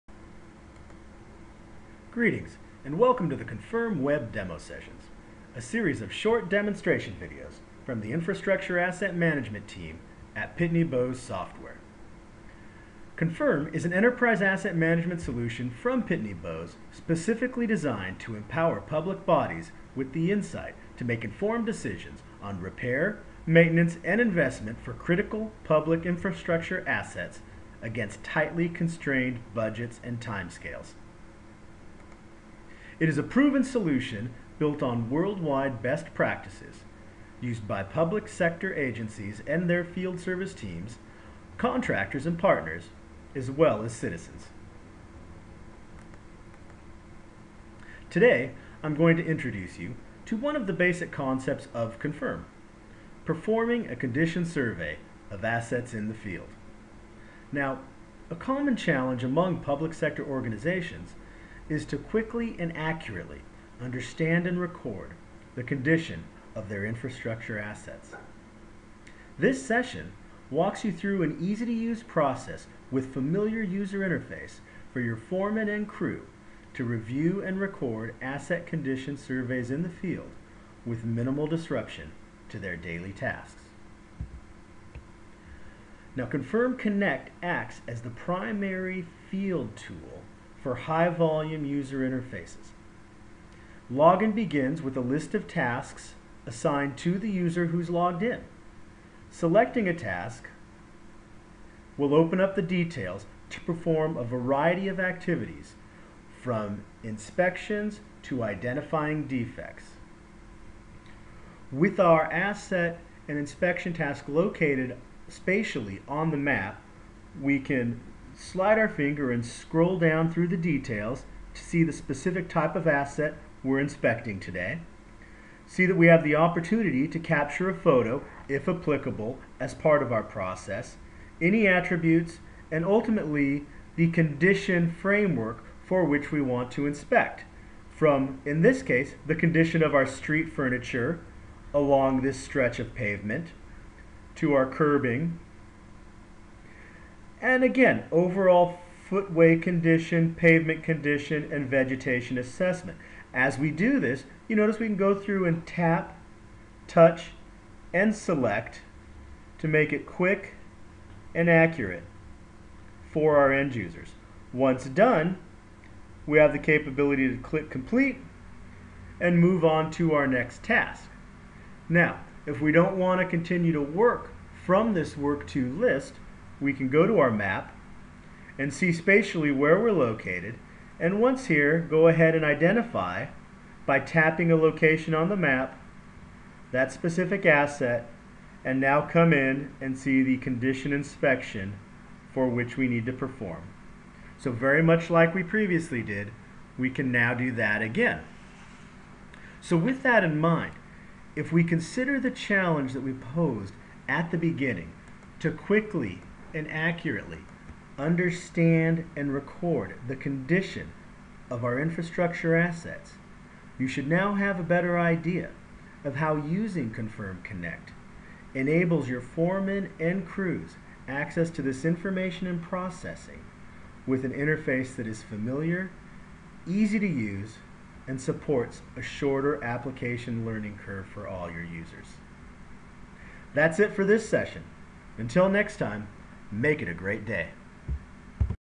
Confirm EAM Software: Asset Capture Survey Demo
In this short video you will see how Confirm EAM software allows public entities to review and record the condition of their infrastructure assets in the field.